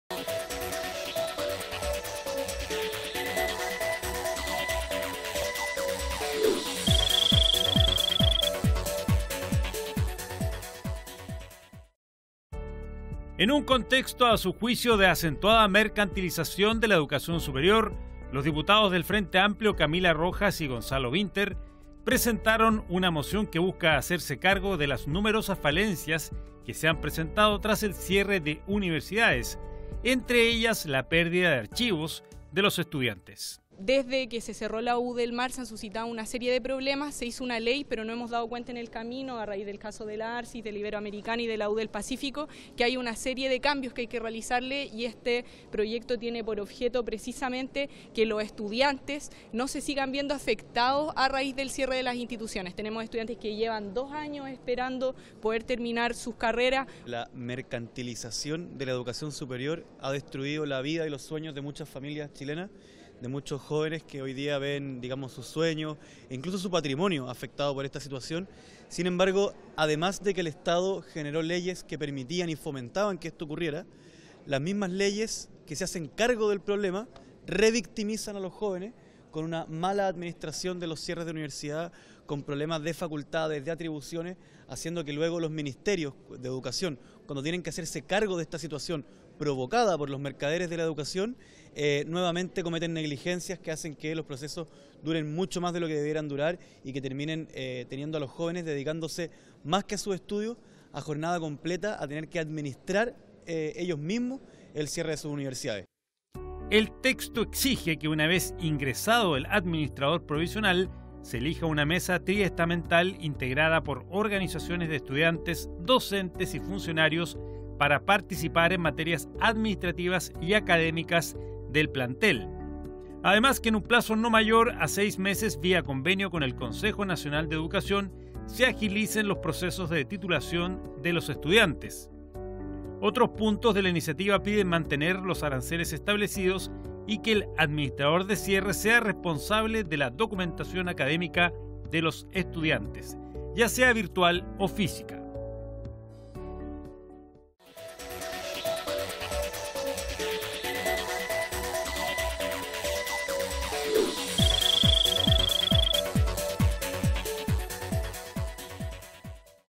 Parlamentarios presentan moción que modifica ley sobre cierre de universidades
Tomando en consideración las experiencias y dificultades que han tenido los estudiantes de distintas universidades que han tenido que cerrar sus puertas, los diputados del Frente Amplio Camila Rojas y Gonzalo Winter presentaron un proyecto que busca modificar la ley sobre administración provisional y cierre de instituciones de educación superior.